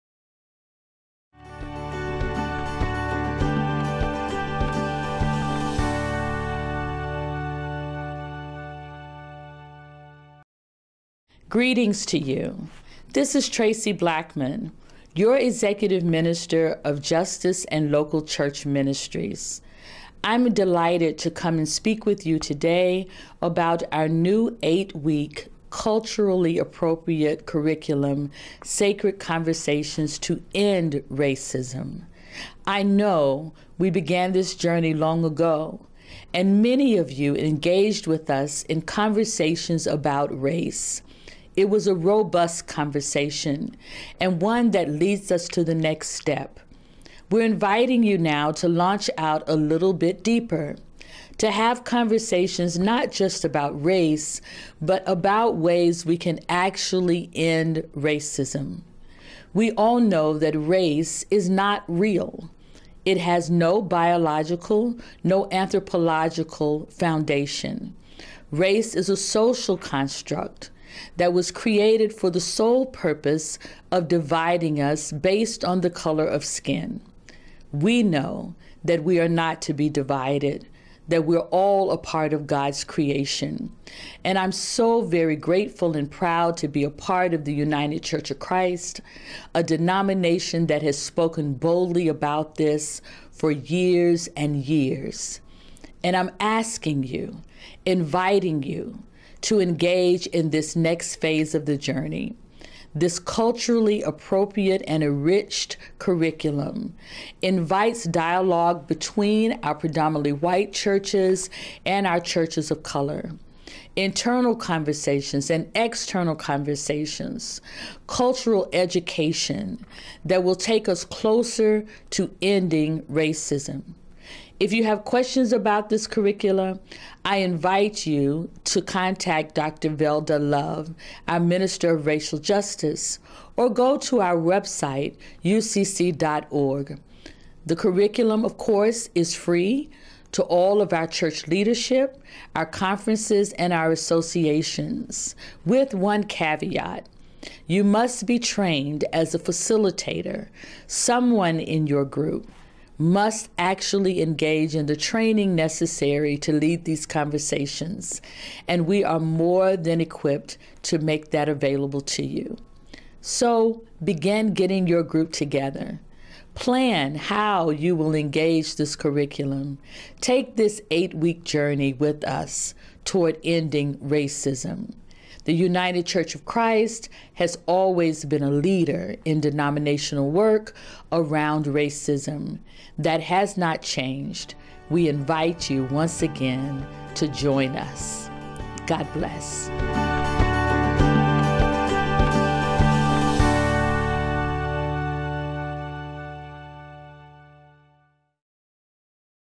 The Myth Called Race
United Church of Christ Executive Minister, the Rev. Traci Blackmon, gives her views on racism and the UCC's new curriculum, Sacred Conversations to End Racism.